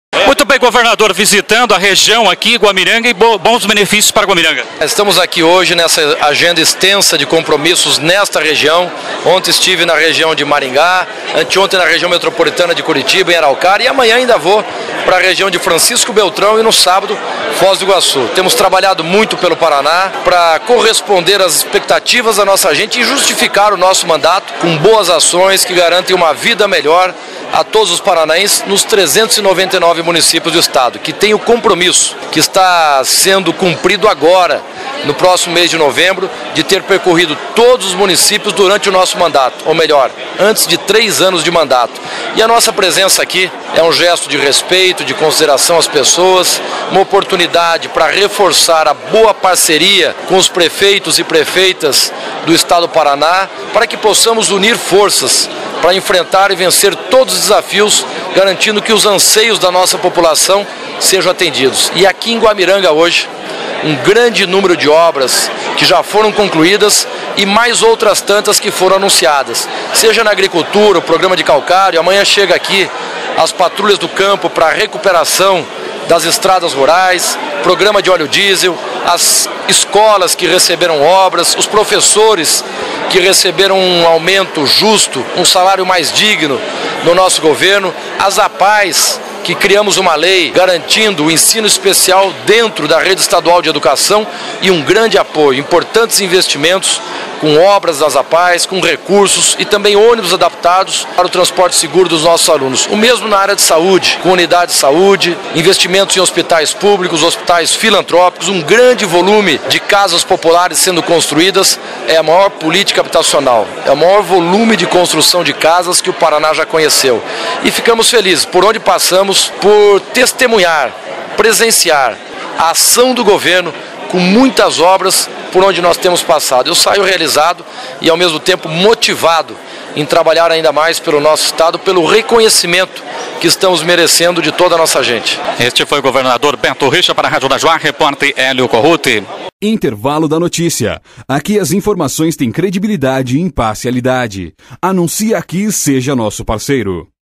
Guamiranga recebe visita de um governador pela 1ª vez - Governador Beto Richa
Durante visita nesta quinta-feira, 24, Beto Richa (PSDB) anunciou investimentos para o município de 8 mil habitantes